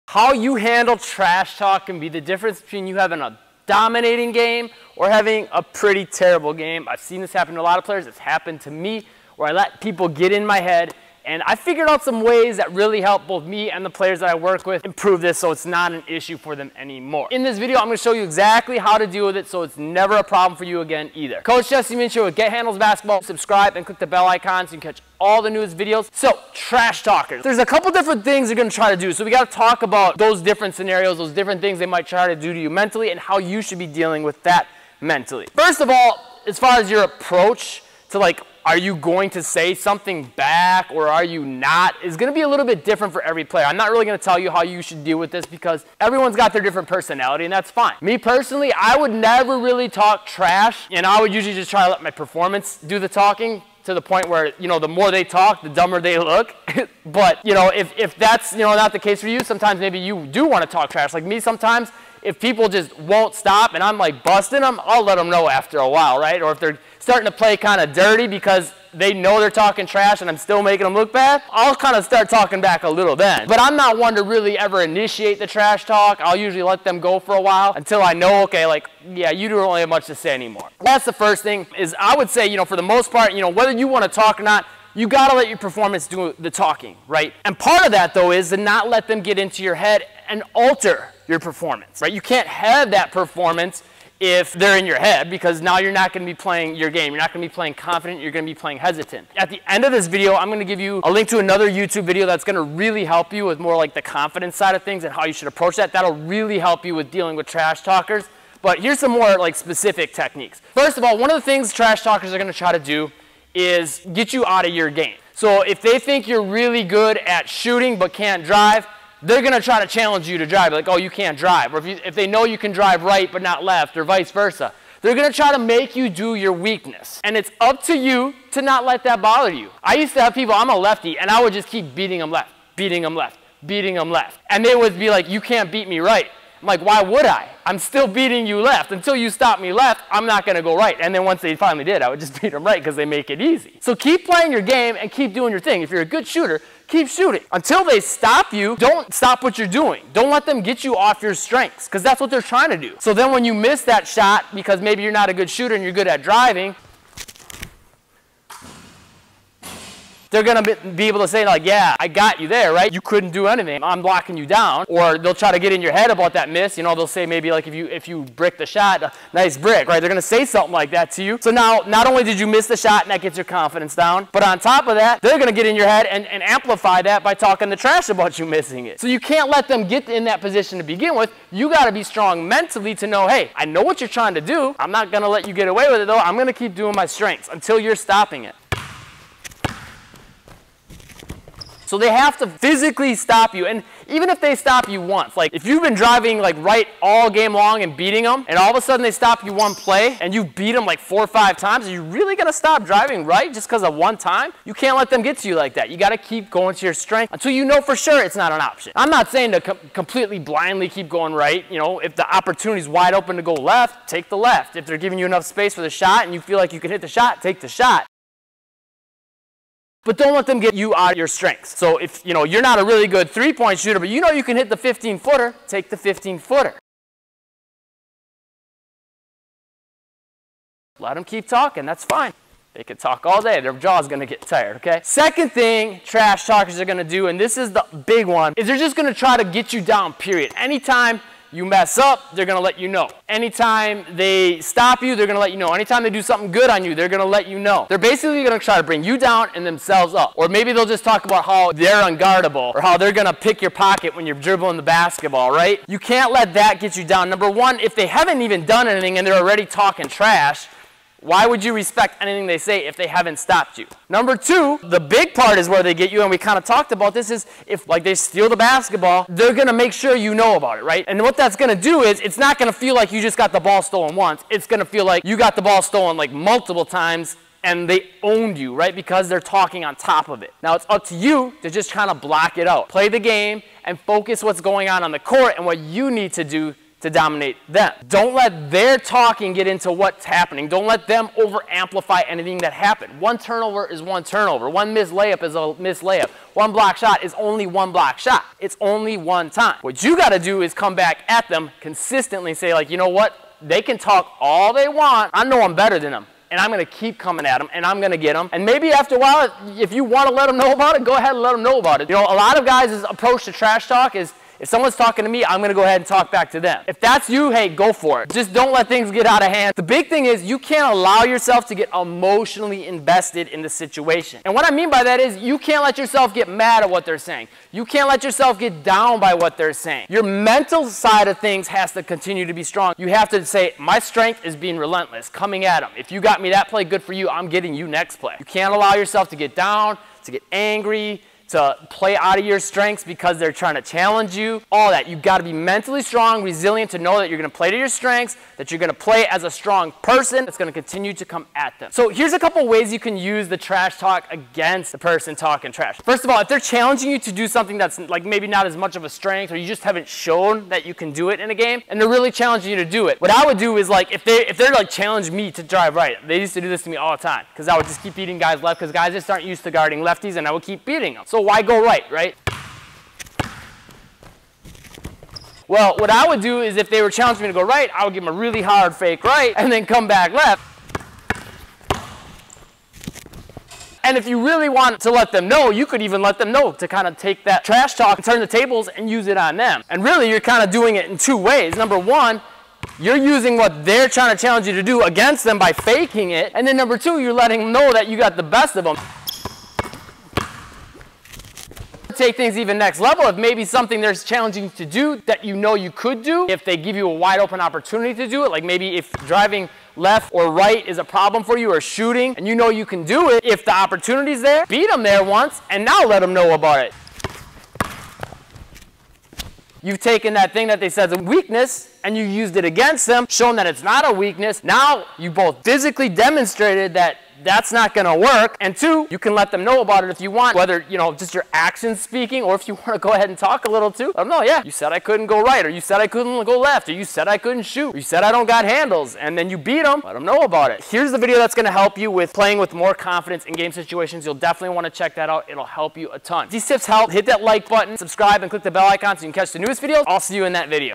TRASH TALK in basketball! WIN The Mental Battle
Trash talk in basketball can mess up your entire game, unless you know how to handle it. In this video you'll learn how to win the mental battle in sports so you come out on top in those 1v1 matchups, they'll feel exposed, and your opponent thinks twice before opening their mouth. Plus you'll discover how to use trash talk AGAINST people!